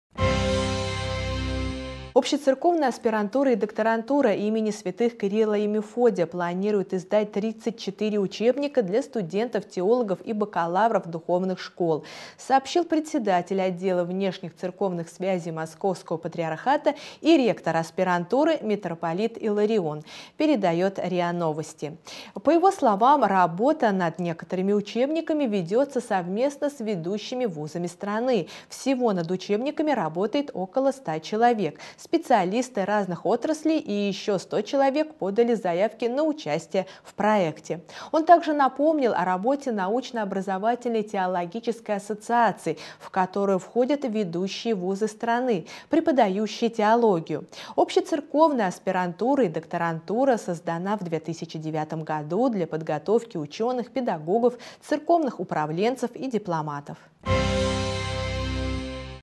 Новые учебники для теологов
Общецерковная аспирантура и докторантура имени святых Кирилла и Мефодия собирается опубликовать тридцать четыре учебника для студентов теологов и бакалавров духовных школ, рассказал председатель отдела внешних церковных связей Московского патриархата и ректор аспирантуры митрополит Иларион, передает РИА-новости.